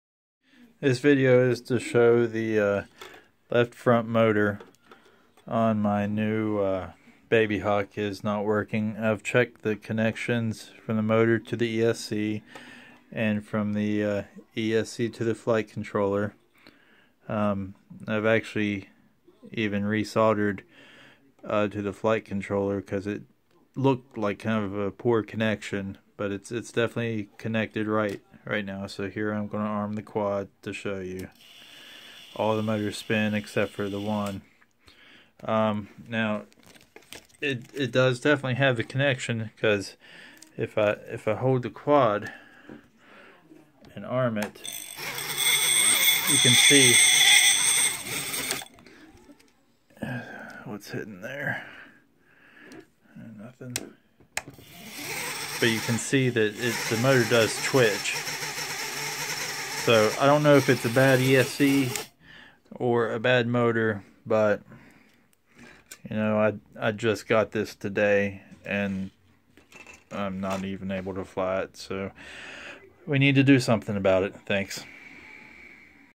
Babyhawk ESC problem
Got my babyhawk and of course I have problem.  It never ends.  This is the video I made for gearbest support.  They've looked at the video but I still dont know what their resolution will be.  I'll update when I find out.

After some more in depth testing, it's definitely a bad ESC.  I can flash the ESC, so the flight controller is able to talk to it.  The start up tones come through the motor, so the connection to the motor is good, but when you do the motor test in BLheli, the motor doesn't spin, it did spin once, but then stopped again.